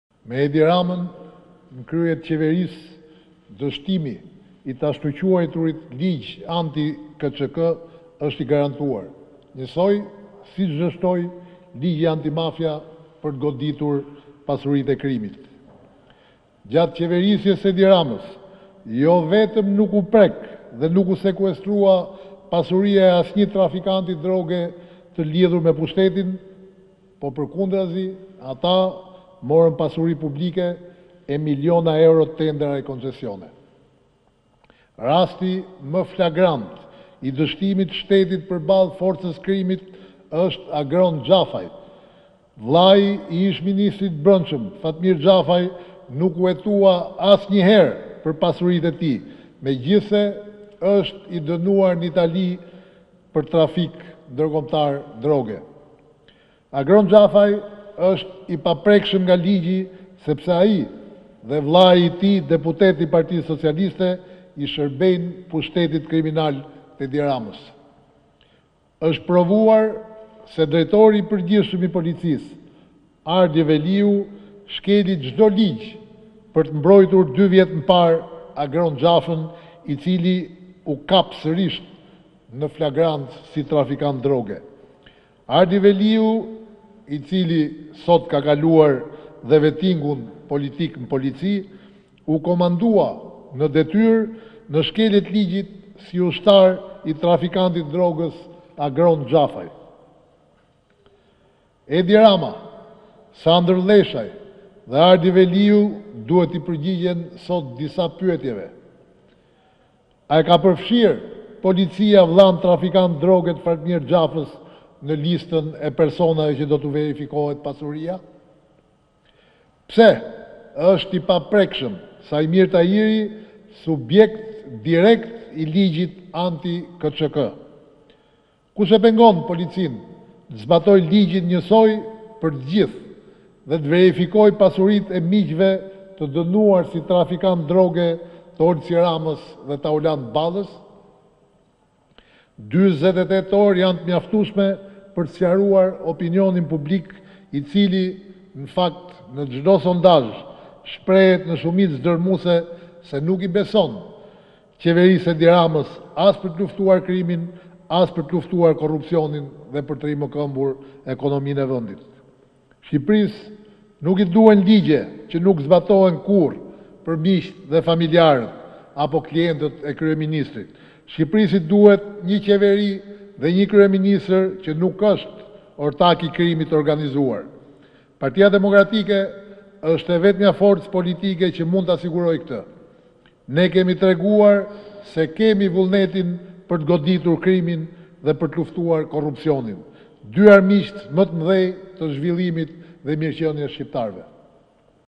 Report TV - Aksioni anti-KÇK-së, Paloka: A do i verifikohet pasuria vëllait të Fatmir Xhafës?
TIRANE-N/kryetari i PD-së, Edi Paloka akuzon edhe njëherë kryeministrin Edi Rama për miratimin e aktit normativ anti-KÇK-së. Ai thotë 'gjatë qeverisjes së Edi Ramës, jo vetëm nuk u prek dhe nuk u sekuestrua pasuria e asnjë trafikanti droge, të lidhur me pushtetin, por ata morën pasuri publike, miliona euro tendera dhe koncesione'. Paloka në deklaratën për mediat ngriti tre pyetje për Edi Ramën, Sandër Lleshajn dhe Ardi Veliun.